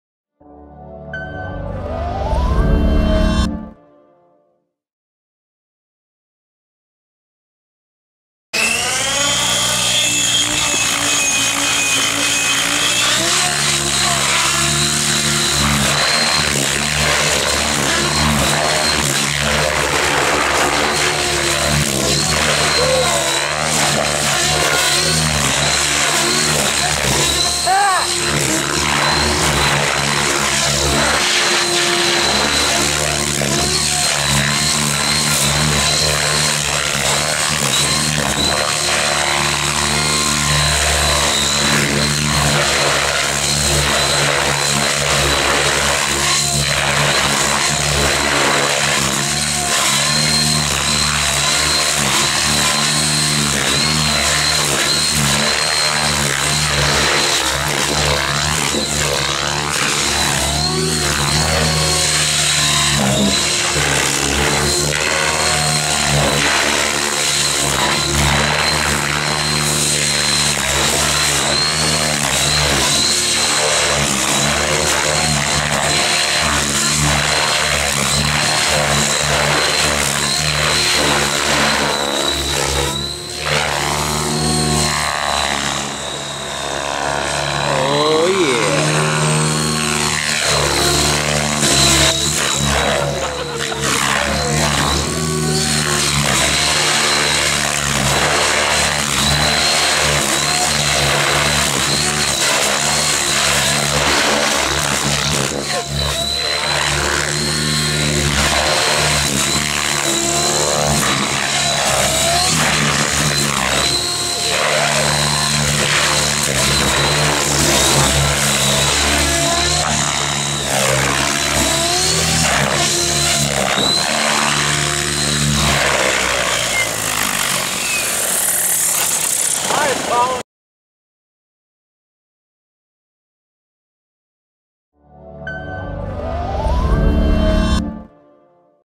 Colin Bell After Hour Parking Lot
Discharging some batteries before traveling back home from IRCHA.